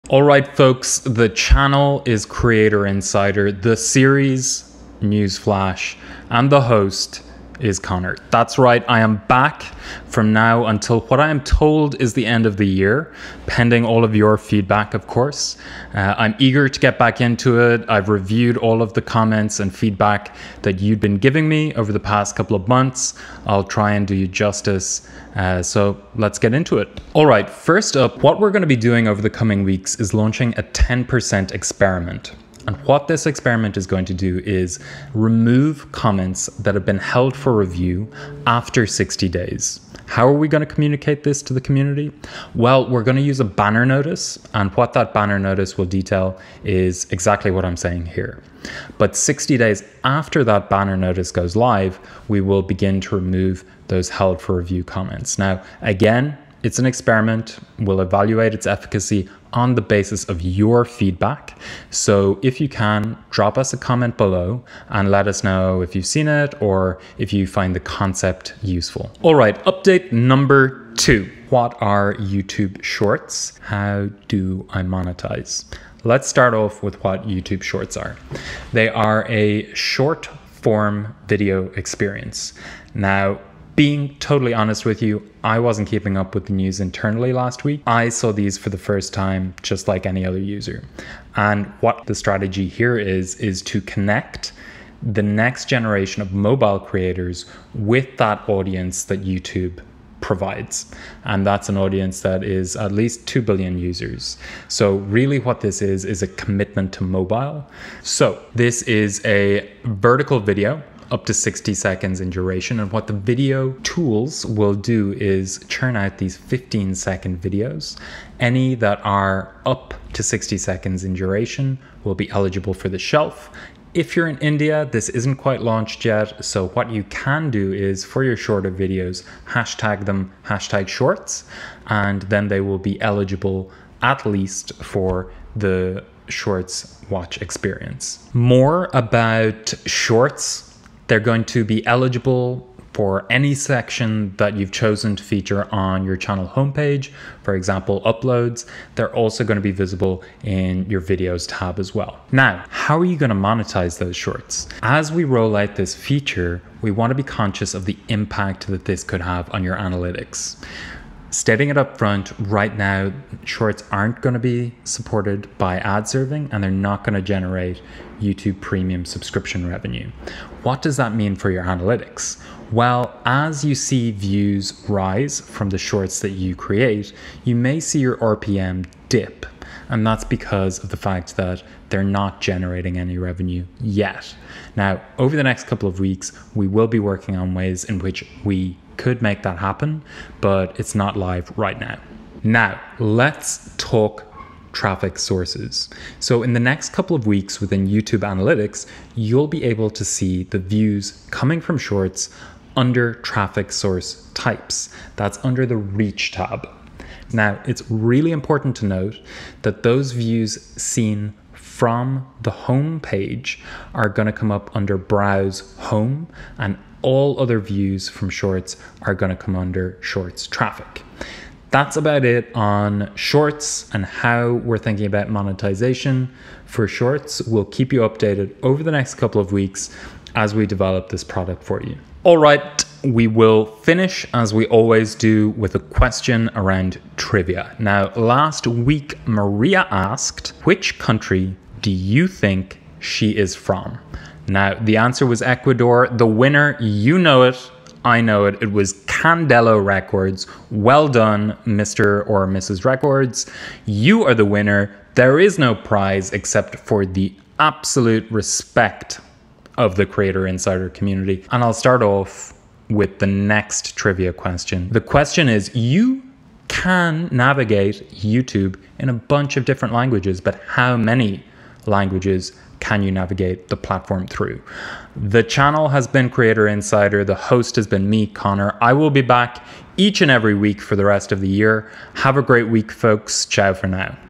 YOUTUBE SHORTS, Held for Review Comments Experiment, and MORE!
Hello Insiders! Today, and through the rest of the year, I am BACK! Today we have updates on YouTube Shorts (What are they? What's the plan for monetization?) as well as an experiment for comments Held for Review.

Chapters:

0:00 Intro
0:26 [Experiment] Removing Held for Review Comments after 60 Days
1:09 YouTube Shorts
3:52 Trivia Winner
4:23 Trivia

👕👚SWAG 🎽☕

100% of the proceeds automatically support Red Cross

Remember, by commenting here, your comment and username may be used in an upcoming video.